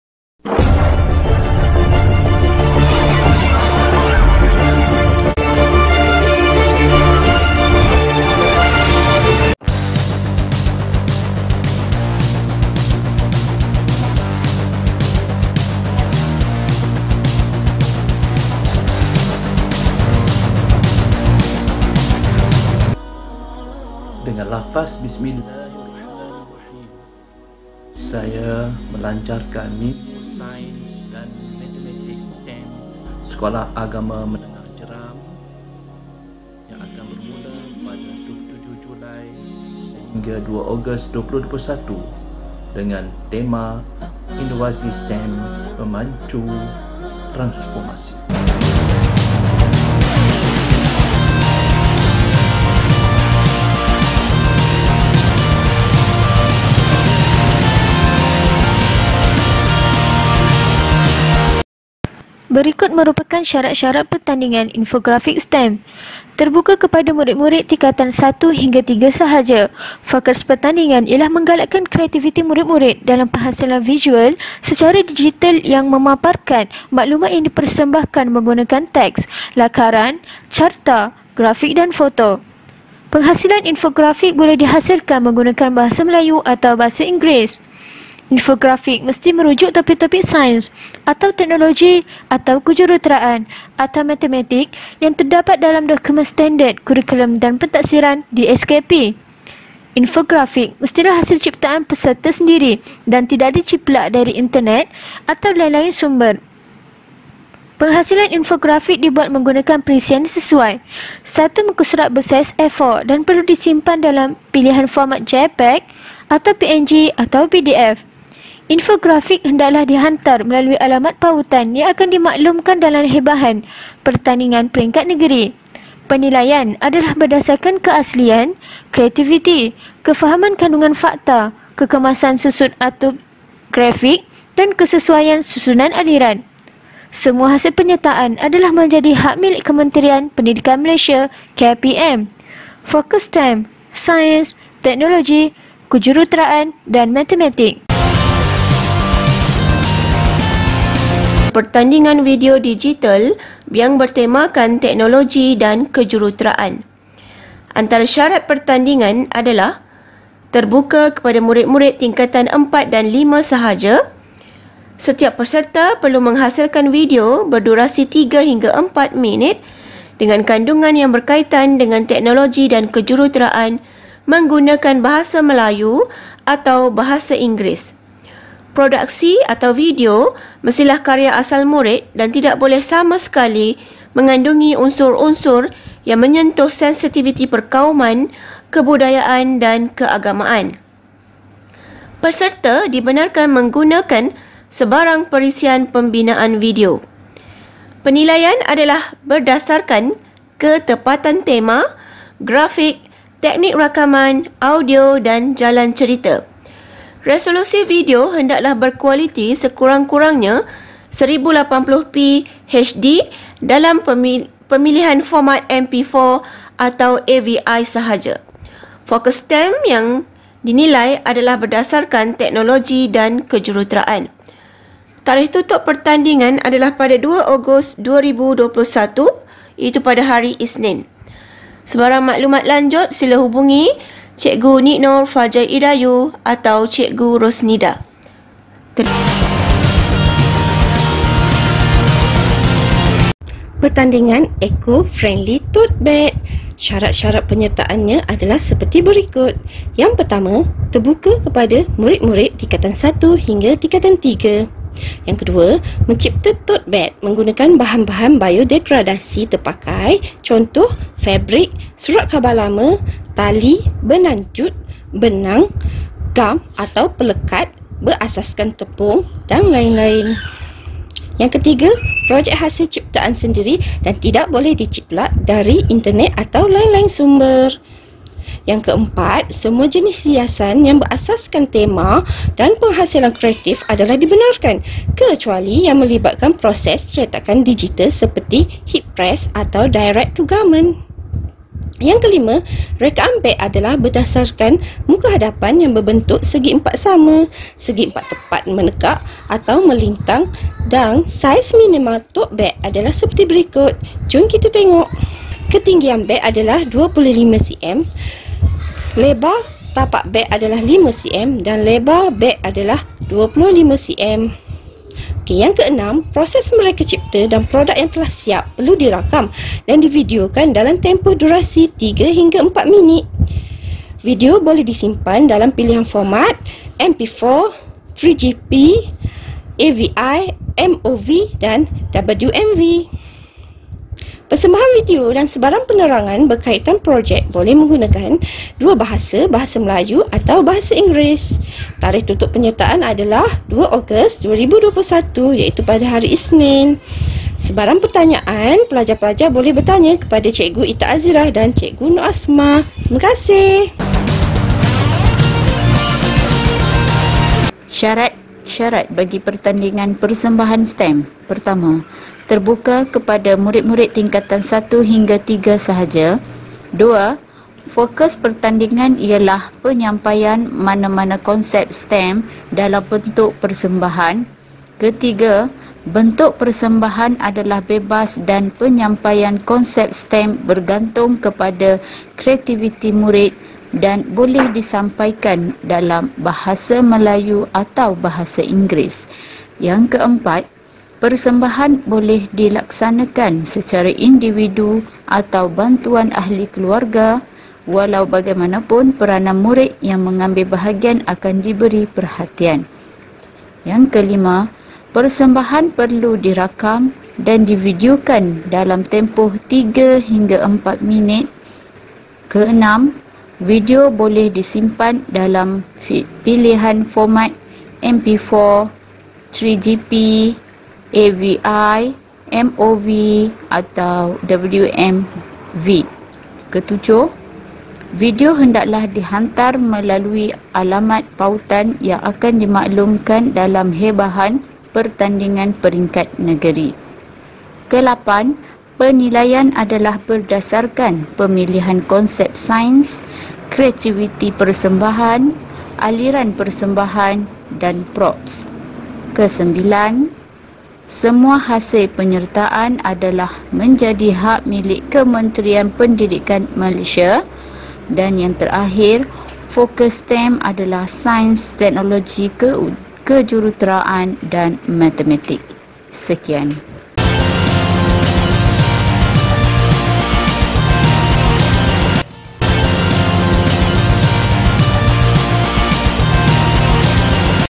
MONTAJ PELANCARAN MINGGU SAINS DAN MATEMATIK (STEM) SEKOLAH AGAMA MENENGAH JERAM
Pelajar-pelajar Sekolah Agama Menengah Jeram digalakkan menyertai pertandingan sepanjang minggu Sains dan Matematik (STEM) 2021 anjuran Bidang Sains dan Matematik untuk meraih sijil PAJSK di peringkat negeri.